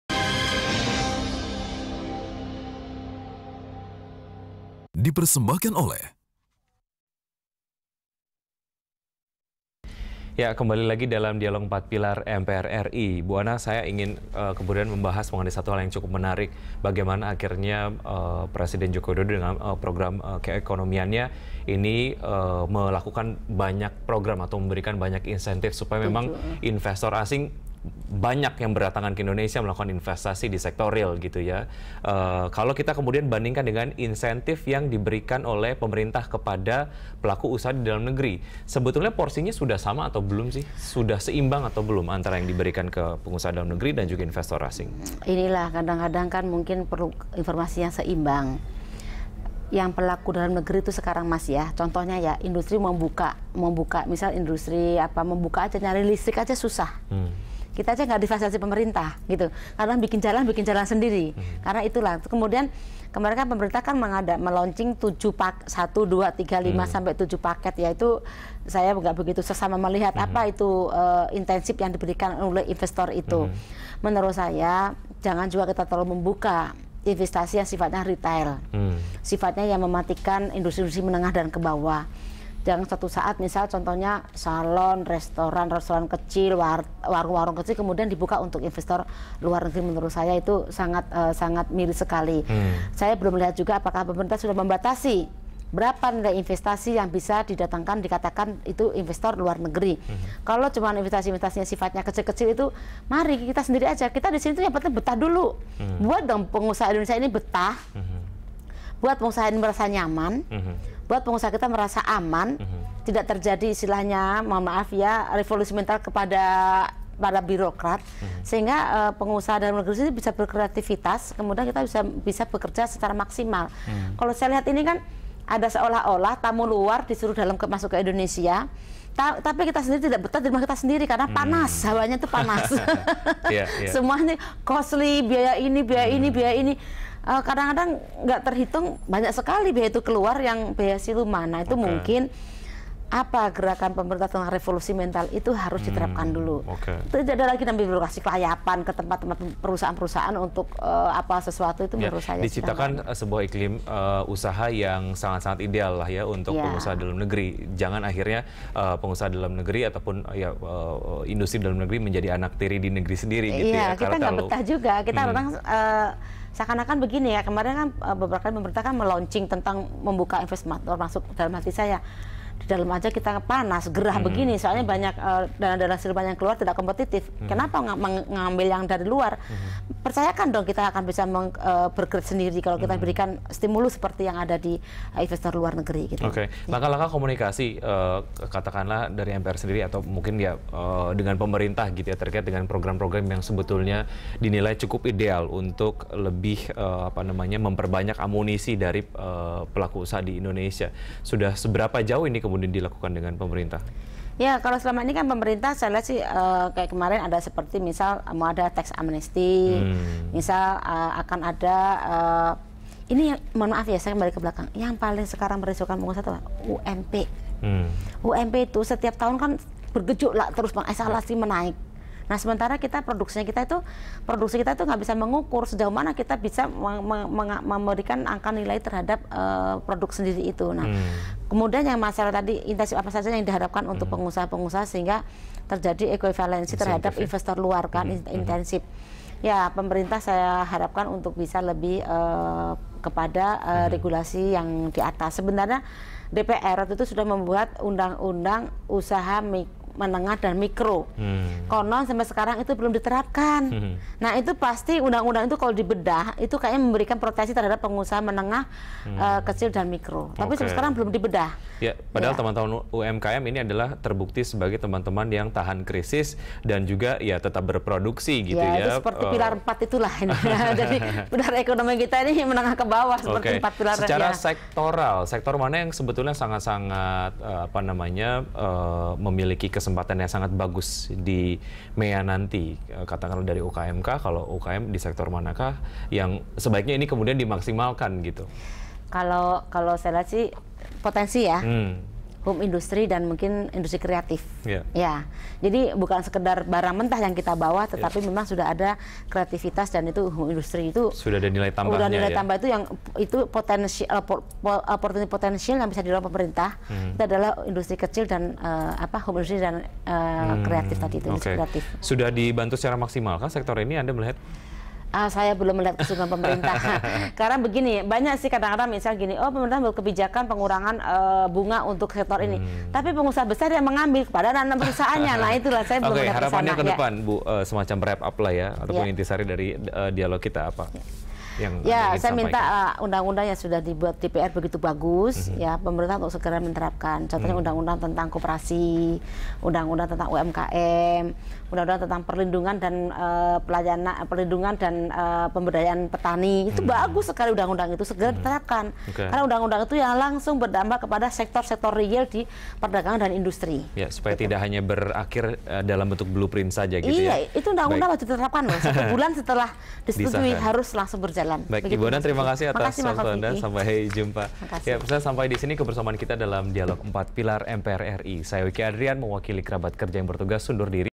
4 Pilar MPR RI: Menuju Masyarakat Ekonomi ASEAN # 3
Tinggal hitungan hari lagi atau mulai 1 Januari nanti, kita akan masuk Masyarakat Ekonomi ASEAN (MEA). Konsekuensinya akan ada pergerakan barang dan tenaga kerja secara bebas antar negara ASEAN. Lantas, sudah siapkah kita menghadapi MEA? Bagaimana strategi kita untuk memenangkan persaingan di MEA? Ikuti dialognya bersama Pimpinan Fraksi PKB MPR RI Anna Mu'awanah.

Facebook.com/BeritaSatuTV
Youtube.com/BeritaSatu